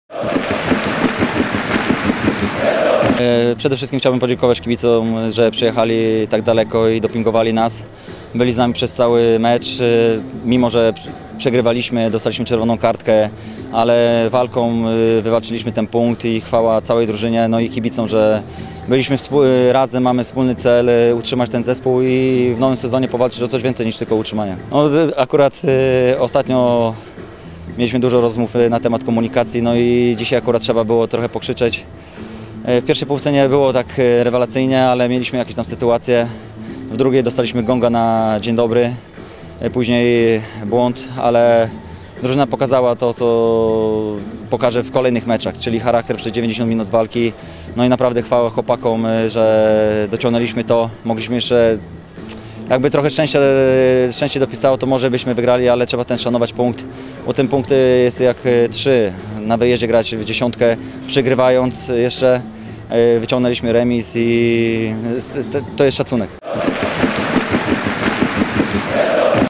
Piotr Celeban ocenia mecz z Koroną
Kapitan Śląska, a zarazem strzelec jedynej bramki dla WKS-u, Piotr Celeban ocenia remis z Koroną Kielce.